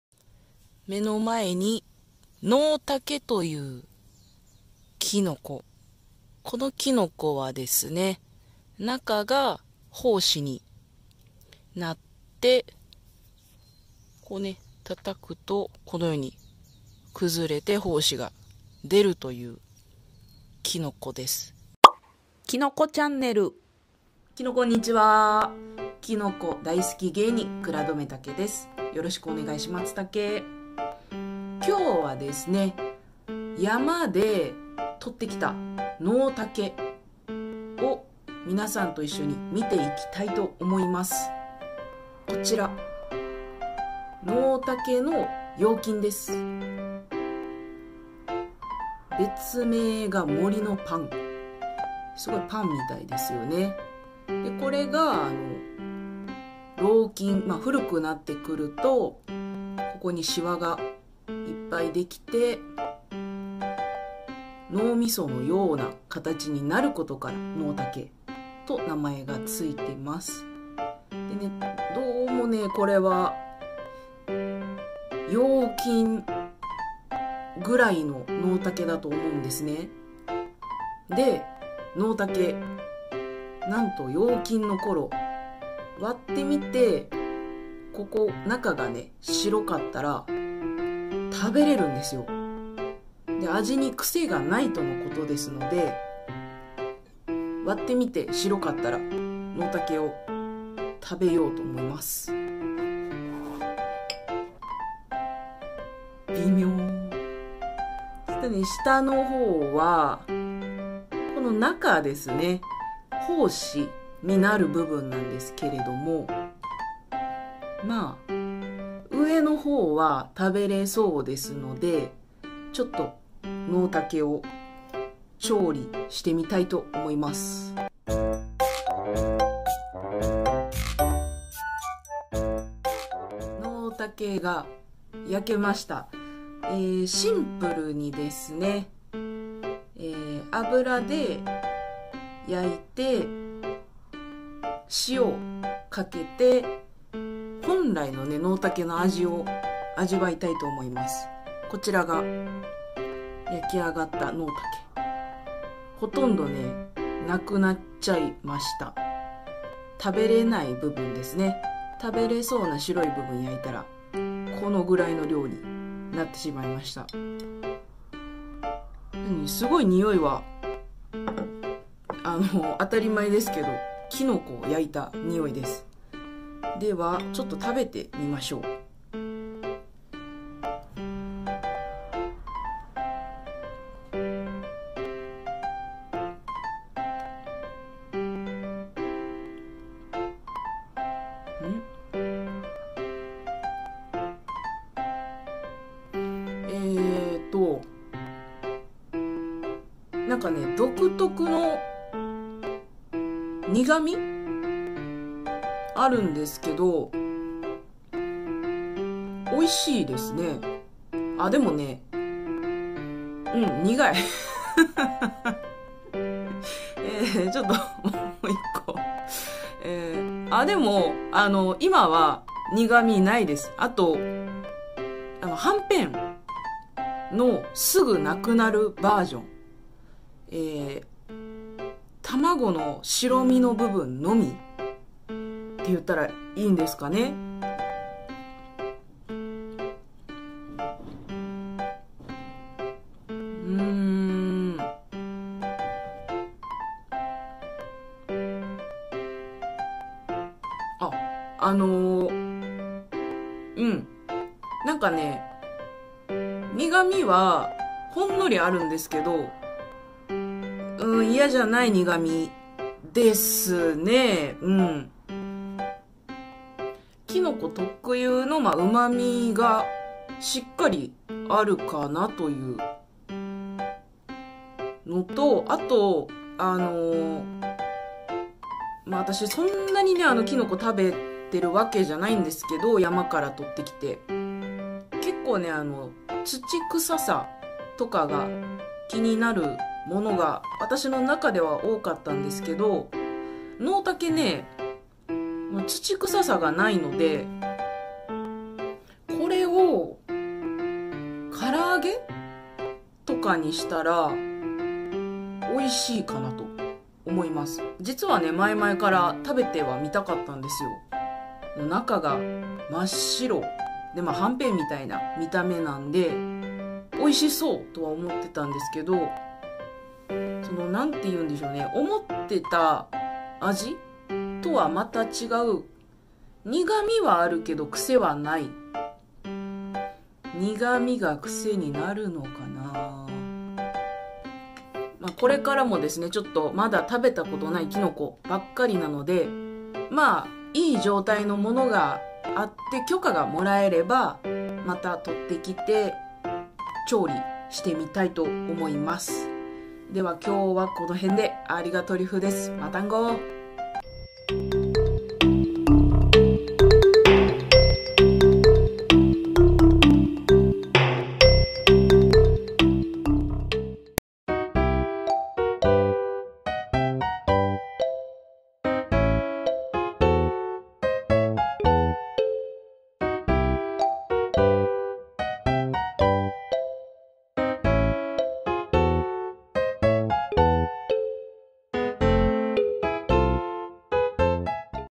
【きのこ試食】きのこ散策 番外編
前々から食べてみたかったきのこを食べてみました( *´艸`)
きのこを食べる為に山にきのこ観察に行ってないので、実はあんまりきのこは食べた事がないです|ω`)笑
この感じだと食レポはかなり先の話になりそうです‹‹\( ´꒳`)/››

ご協力きのこ
🍄ノウタケ